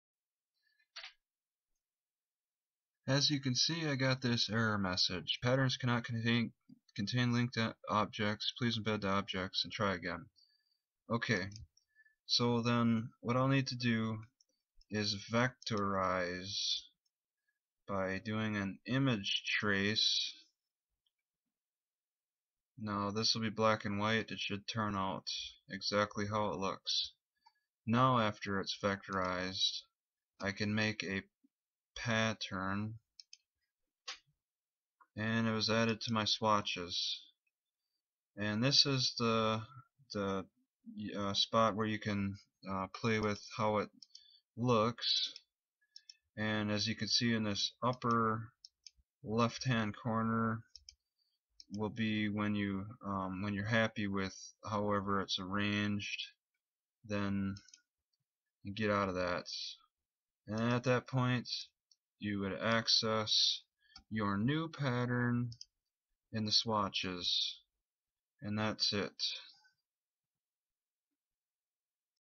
Patterns cannot contain linked object(s). please embed the object and try again
This is just a quick video in case you or I forget why we can't make a pattern immediately. It seems to be a problem with the vectorization option. Just simply vectorize the object so you can make patterns out of it.